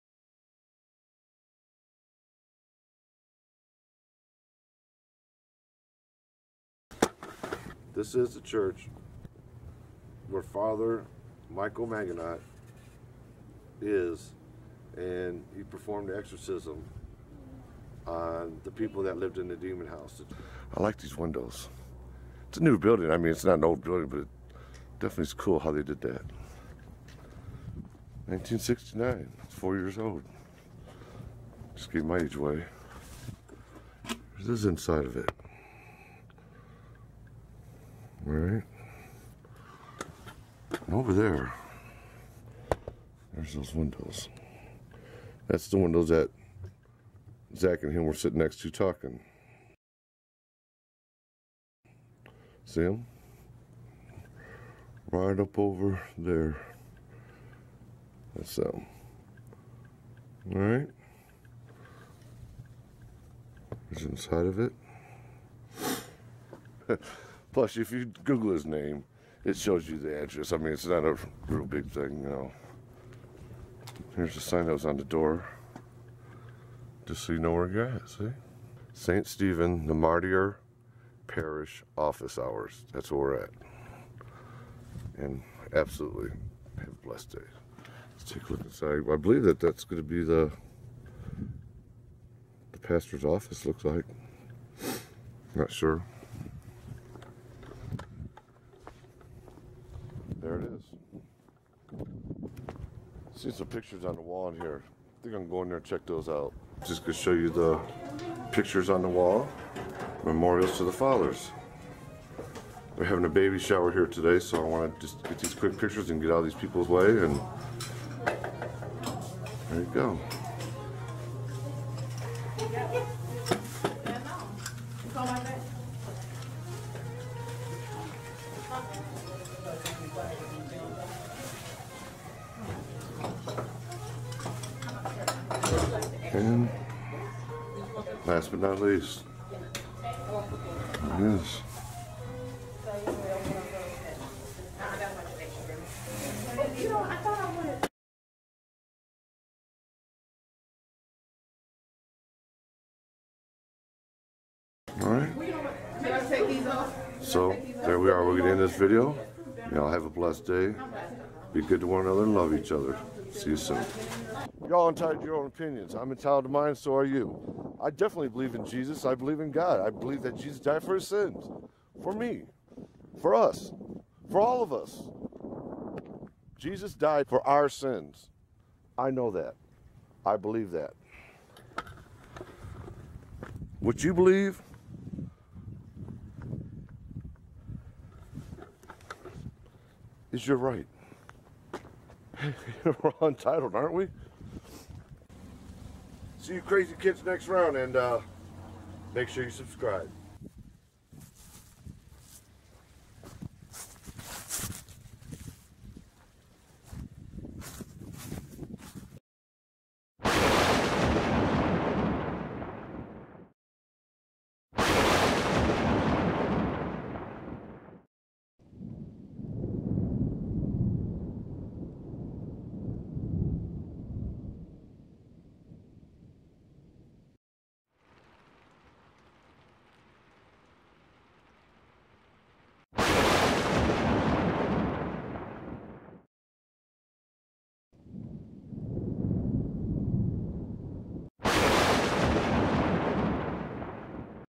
The "DEMON HOUSE" Filming Location - Father Maginot's Church in Merrillville, IN
For three years, he was charge of the Tribunal as Judicial Vicar for the Diocese, then became Pastor of the parish. In 2012, he was given permission by the bishop to be an exorcist for the Ammon family in Gary, Indiana, whose case eventually would receive worldwide notoriety. Here is His Church